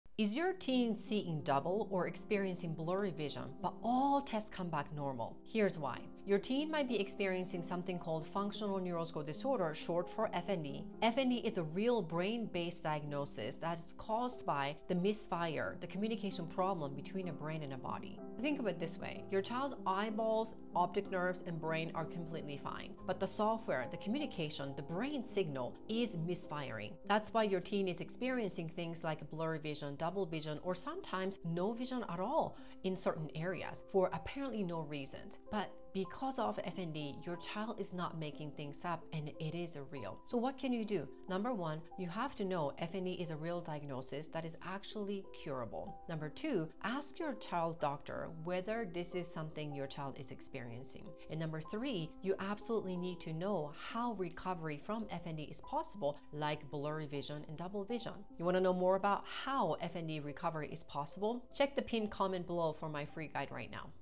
Why Is My Teen Seeing Double?
If your teen is seeing double, has blurry vision, or even sudden vision loss — but all the tests are normal… here’s what might be going on.
It could be Functional Neurological Disorder (FND) — a real brain-based condition where the signals between the brain and body misfire, even though the eyes and brain structure are healthy.
FND is real. And yes — vision symptoms can be part of it. Recovery is possible.